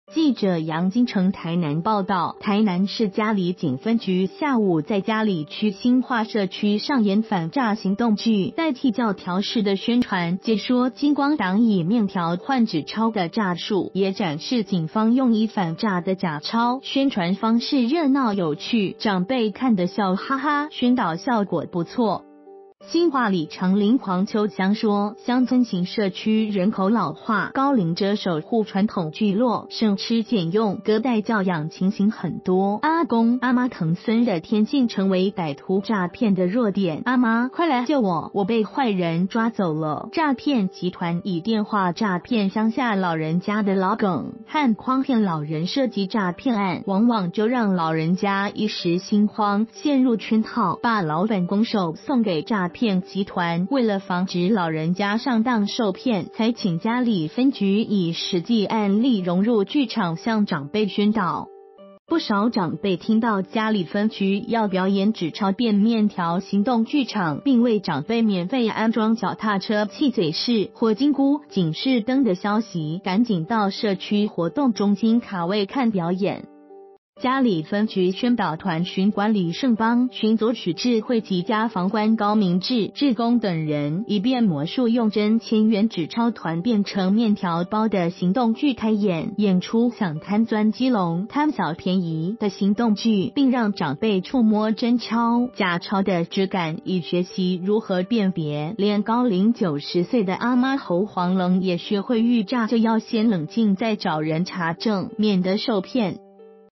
表演紙鈔變麵條的詐術 台南佳里警反詐行動劇有趣
〔記者楊金城／台南報導〕台南市佳里警分局下午在佳里區興化社區上演反詐行動劇，代替教條式的宣傳，解說金光黨以麵條換紙鈔的詐術，也展示警方用以反詐的假鈔，宣傳方式熱鬧有趣，長輩看得笑哈哈，宣導效果不錯。

發表評論評論如下。